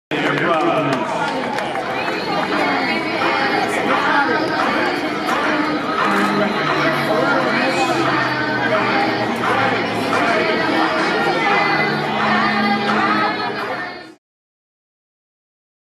Bat Dog makes A-Rod wait
TRENTON - Wednesday, May 25, 2016 New York Yankees' designated hitter Alex Rodriguez comes to the plate on his 2nd day of a rehab assignment (hamstring) playing with the Double-A Trenton Thunder vs the New Hampshire Fisher Cats, while Thunder Bat Dog Derby picks up the bat. (Michael Mancuso | For NJ.com )